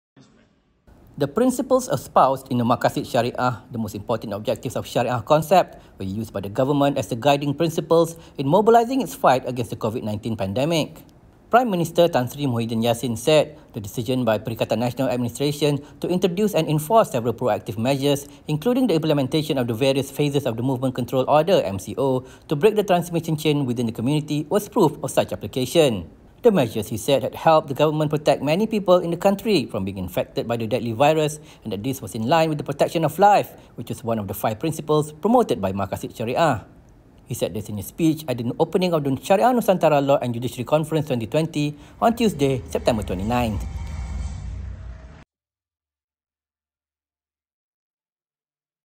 PM: Govt utilises Maqasid Syariah principles in fight against Covid-19
The principles espoused in the Maqasid Syariah (the most important objectives of Syariah) concept were used by the government as the guiding principles in mobilising its fight against the Covid-19 pandemic.

Prime Minister Tan Sri Muhyiddin Yassin said the decision by Perikatan Nasional administration to introduce and enforce several proactive measures including the implementation of the various phases of the Movement Control Order (MCO) to break the transmission chain within the community was proof of such application.

The measures, he said, had helped the government protect many people in the country from being infected by the deadly virus and that this was in line with the protection of life which was one of the five principles promoted by Maqasid Syariah.

He said this in his speech at the opening of the Syariah Nusantara Law and Judiciary Conference 2020 on Tuesday, September 29th.